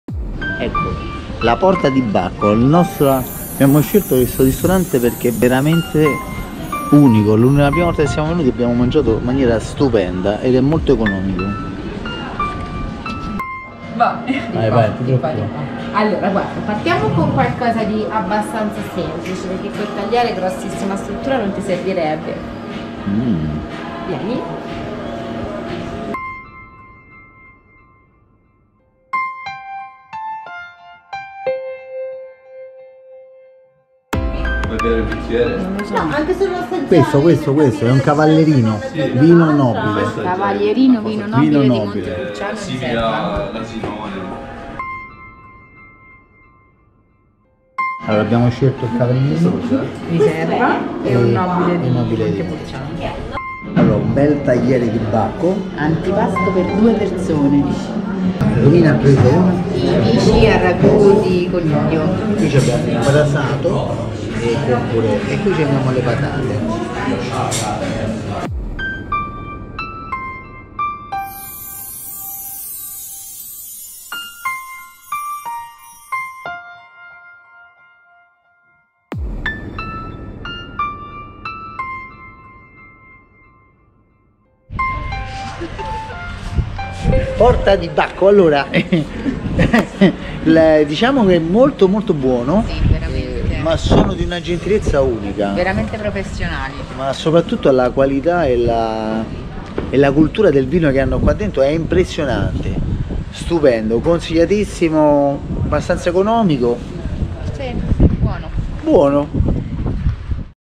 Ristorante Porta di Bacco Montepulciano Siena Italy
* * * * * Stelle - Rapporto qualità/prezzo: Eccellente
Situato al centro di Montepulciano, ristorante tipico toscano!!! Staff molto disponibile e preparato, tempi e cibo perfetti!!! Il vino, scelto direttamente nell'enoteca del ristorante,  abbinato alla perfezione con le portate scelte!!! Prezzi adeguati a quello che si mangia!!! Consigliatissimo!!!

Photos and info on our Facebook page
Pagina Facebook Montepulciano